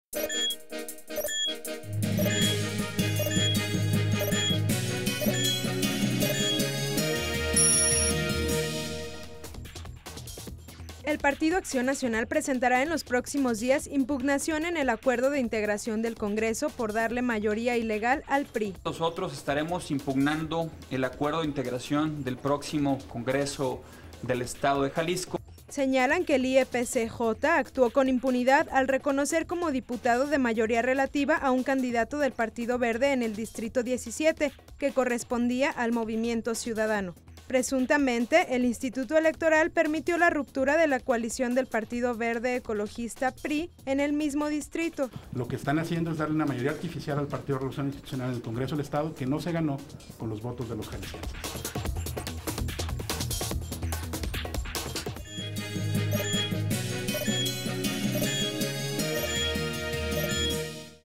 Corte 44 - PAN acusa al IEPCJ de otorgar mayoría en el Congreso al PRI -Julio 9 de 2012
El Partido Acción Nacional presentará en los próximos días impugnación en el acuerdo de integración del Congreso por darle mayoría ilegal al PRI.

Informe Celia Niño